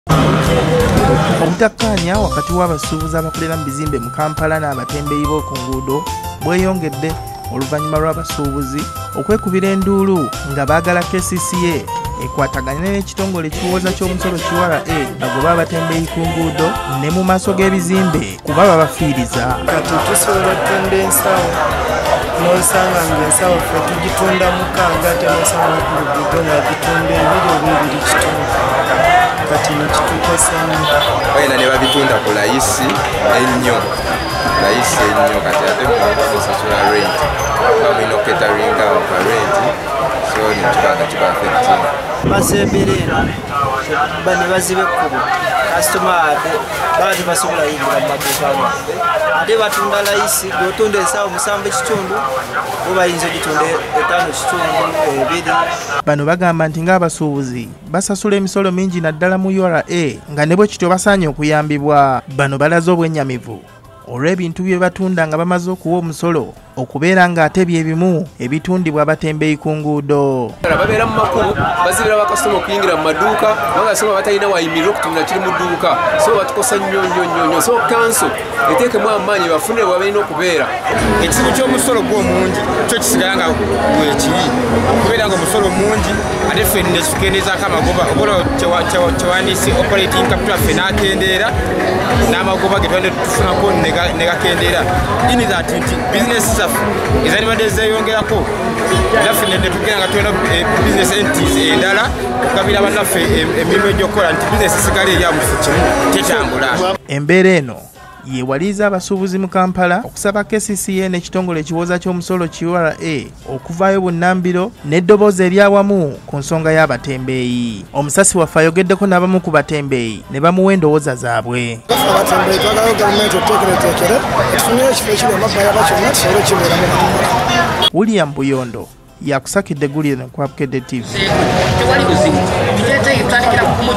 Ttuntu: Abasubuuzi balajaana kwa nsonga y'abatembeeyi ku nguudo.
Obutakanya mu basubuuzi abakolera mu bizimmbe by’omukampala nabatembeeyi kweyongedde abasubuuzi bwebekubidde endulu nga bagala kcca akwatagane nekitongole ekiwooza omusolo ki URA bagobe abatembeeyi ku nguudo .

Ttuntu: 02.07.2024
Sumayiyah Nakato